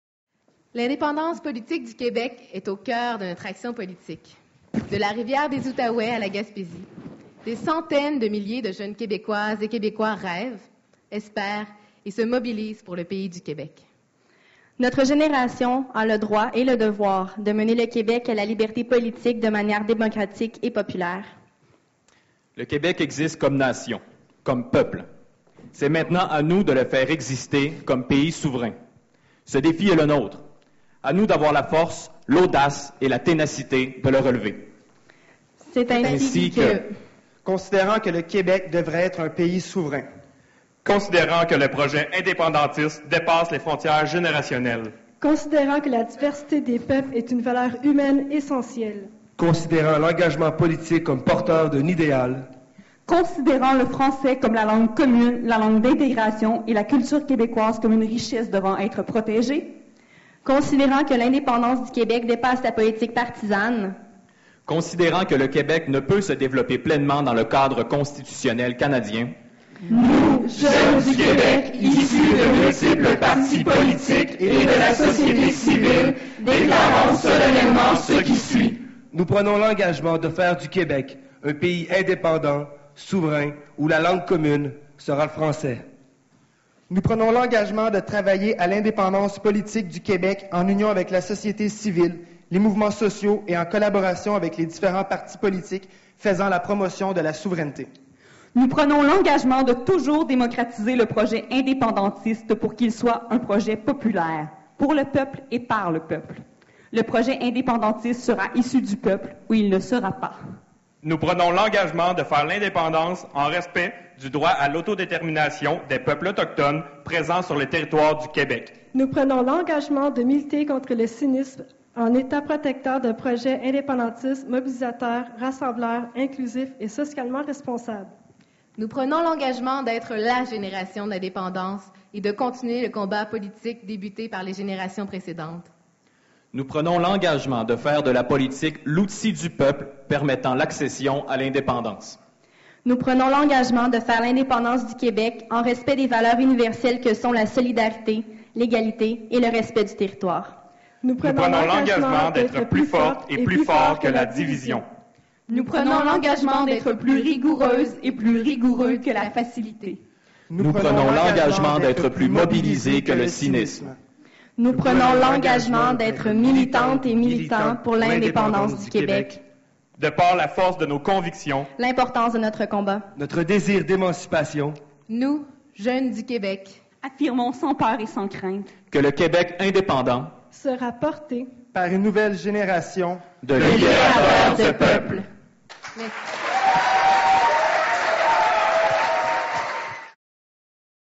Grande déclaration des jeunes souverainistes
Montréal, 13 août 2010 - C'est avec conviction que des représentantes et représentants des ailes jeunesse du Parti Québécois, de Québec solidaire et du Bloc Québécois ont fait la lecture de la Grande déclaration des jeunes souverainistes, marquant ainsi le coup d'envoi de l'Université d'été des jeunes souverainistes. Ils ont réaffirmé l'importance pour les Québécoises et Québécois de raviver le projet souverainiste pour enfin se donner un pays.